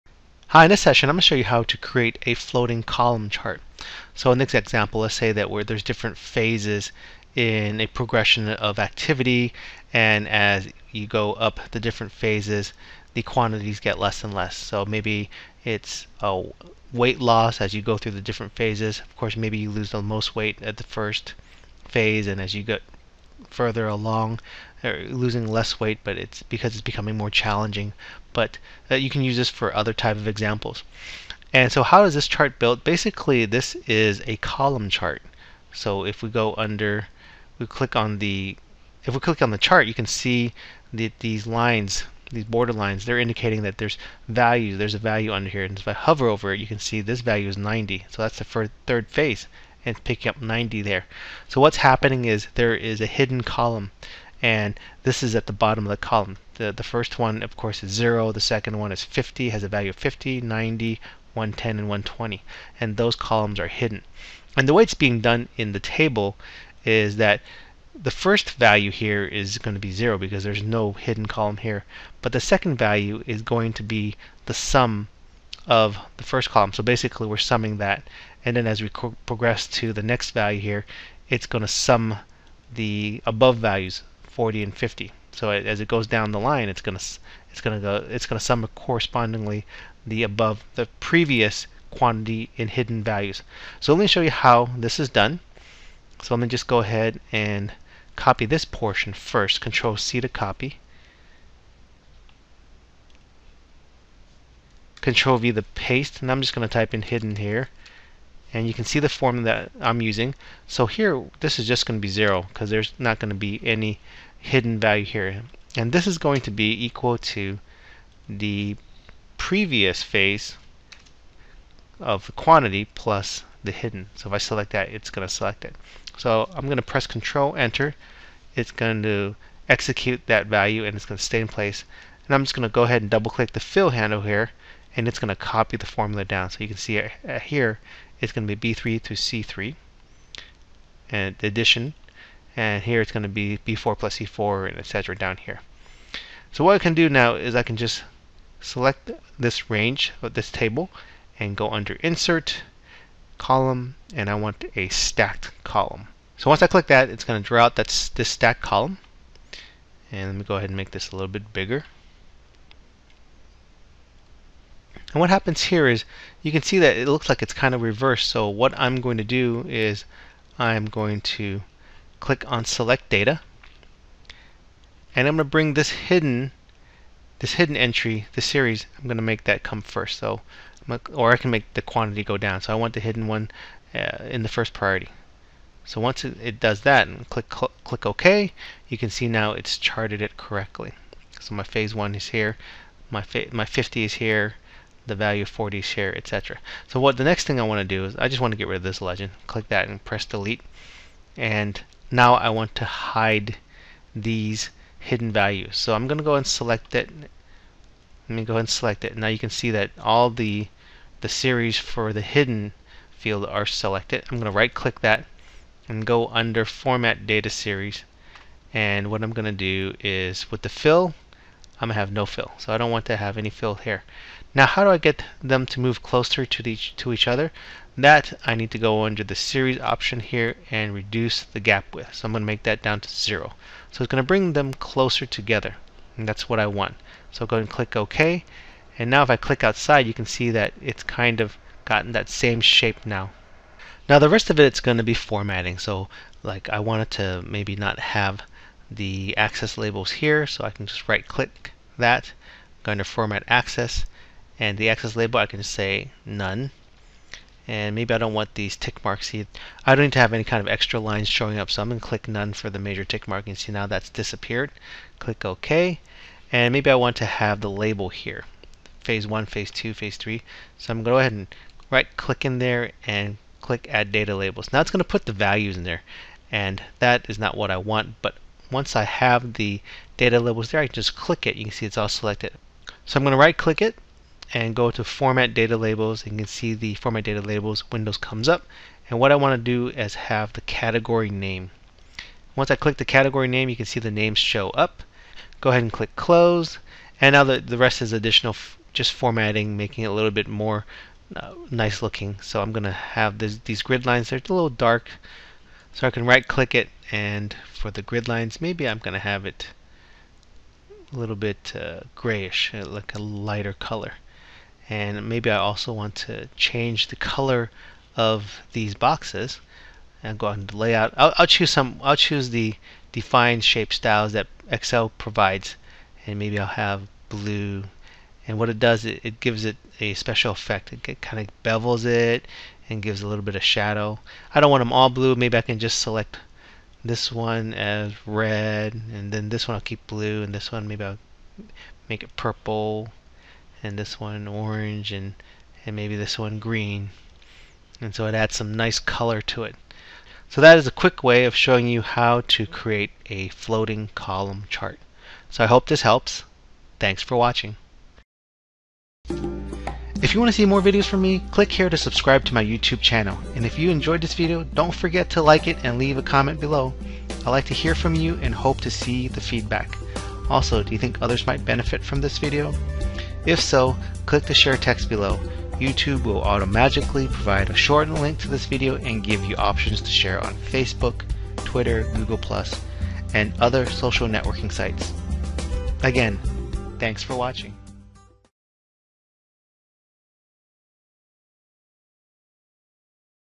Create a Floating Column Chart in Excel
Column charts are a standard chart in Excel to present data in a more visual way.  A neat trick is to show specific data in the column chart and do comparisons between series.  See this video to learn a quick way on how to create a floating column chart in Excel .